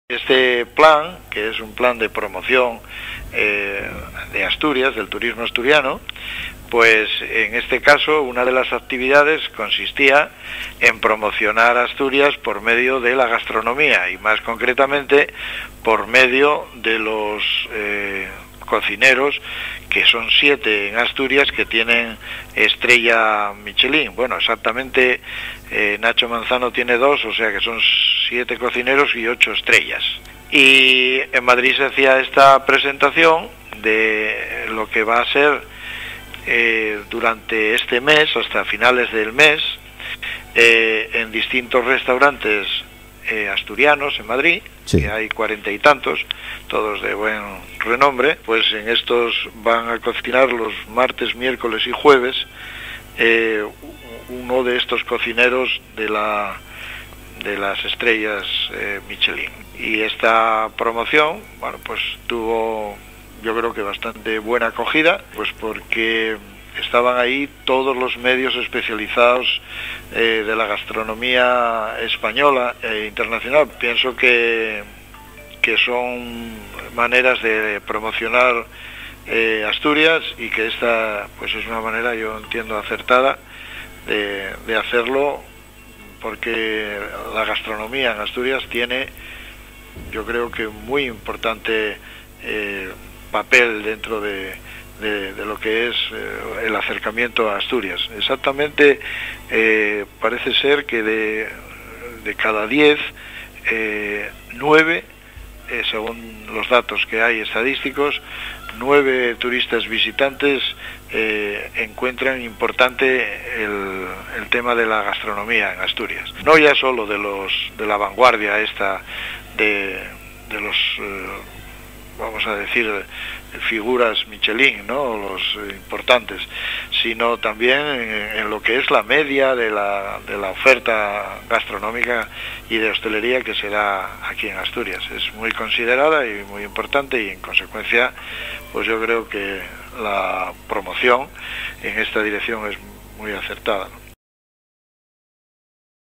ORT FACC Turismo Gastronómico 11-03-2014
"Saboreando Estrellas" es el nombre del evento de promoción turística, organizado por la FACC, el Principado y el Gobierno del Estado, cuyo objetivo es la promoción gastronómica de Asturias. Al acto asistió el presidente de la FACC; Ignacio García Palacios, y ha explicado en Ser Occidente que los siete cocineros asturianos con Estrellas Michelín, cocinarán esta semana en restaurantes asturianos en Madrid. "Es una experiencia muy positiva", ha señalado, "con gran repercusión en los medios de comunicación especializados". También ha recordado la importancia que los visitantes conceden a la gastronomía asturiana.